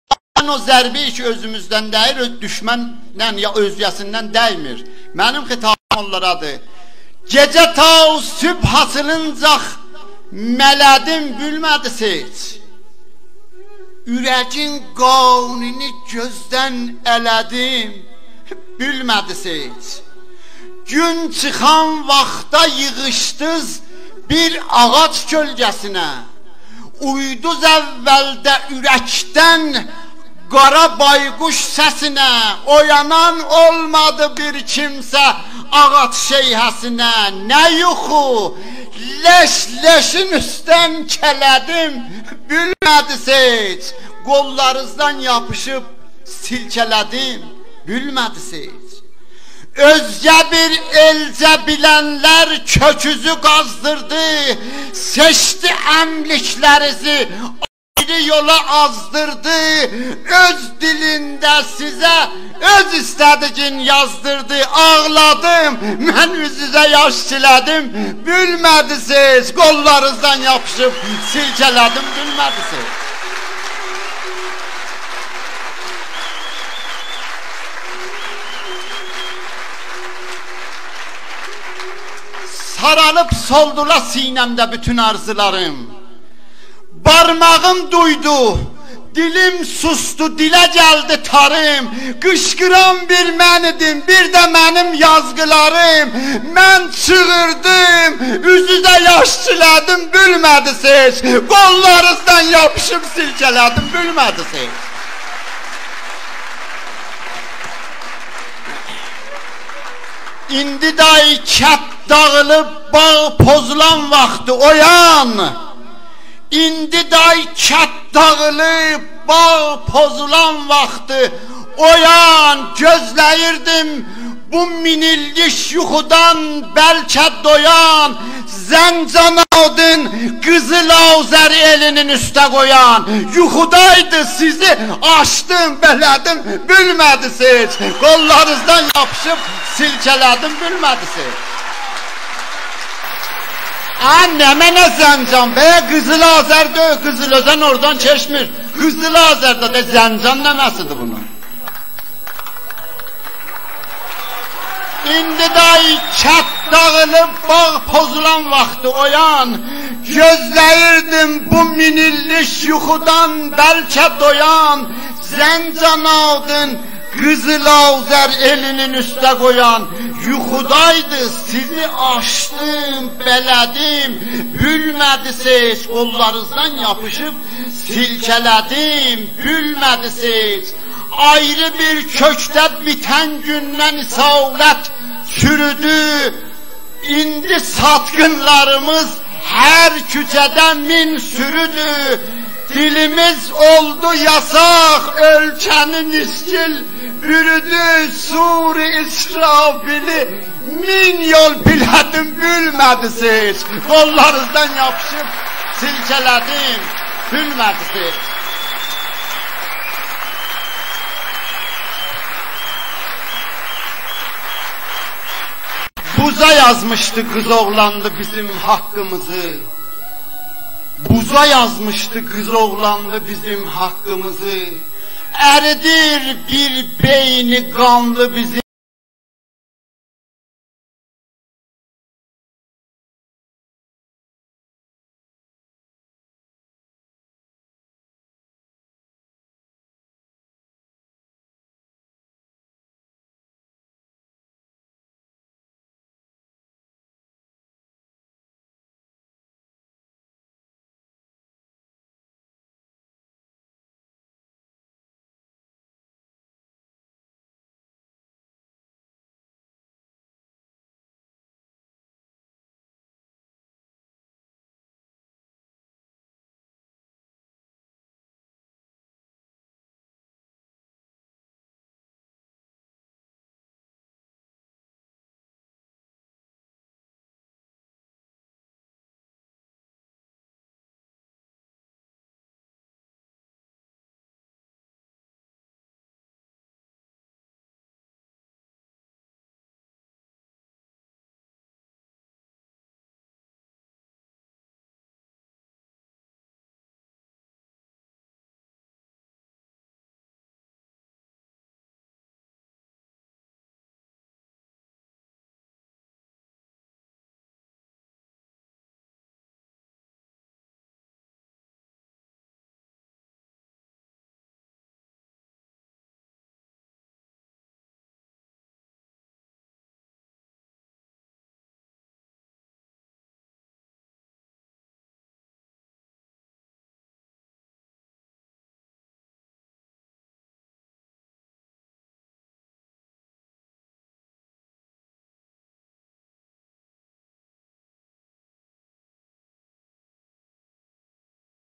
OYAN
Qoşar : Hüşəng Cəfəri
Qoşma : OYAN
Hüşəng Cəfəri bəy Azərbaycanımızın Zəngan şəhərindədir. Hüşəng bəyin Azərbaycanımızın bütövlüyü uğrunda çoxlu qoşmaları və çabaları olmuşdur və hələdə bu yolda bütün gücü ilə çaba göstərir.

Qoşma :

Gecə ta sübh açılınca mələdim bilmədiz heç,
Ürəyin qanını gözdən ələdim bilmədiz heç.

Gün çıxan vaxtda yığışdız bir ağac kölgəsinə,
Uyduz əvvəldə ürəkdən qara bayquş səsinə,
Oyanan olmadı bir kimsə “Ağ At” şeyhəsinə,
Nə yuxu!, leş-leşin üstə kələdim bilmədiz heç,
Qollarınızdan yapışıb silkələdim bilmədiz heç.

Özgə bir elcə bilənlər, kökünüzü qazdırdı,
Seçdi əmliklərinizi ayrı yola azdırdı,
Ayrı dildə sizə öz istədiyin yazdırdı,
Ağladım, mən üzünüzə yaş çilədim bilmədiz heç,

Saralıb soldular sinəmdə bütün arzılarım,
Barmağım doydu, dilim soldu, dilə gəldi tarım,
Qışqıran bir mən idim bir də mənim yazqılarım,
Sizi fəryad eləyib çəngələdim bilmədiz heç,
Qollarızdan yapışıb silkələdim bilmədiz heç.

İndi day kənd dağılıb bağ pozulan vaxdı oyan,
Gözləyirdim bu min illik yuxudan bəlkə doyan,
“Zəncan” adın qızıl Azər elinin üstə qoyan,
Yuxudaydız, sizi açdım, bələdim bilmədiz heç,

Ayrı köklərdə bitən gündən əsalət çürüdü,
İndi satqınlarımız hər küçədən bir sürüdü,
Dilimiz oldu yasaq ölkəni nisgil bürüdü,
Sur-i İsrafili min yol pilədim bilmədiz heç,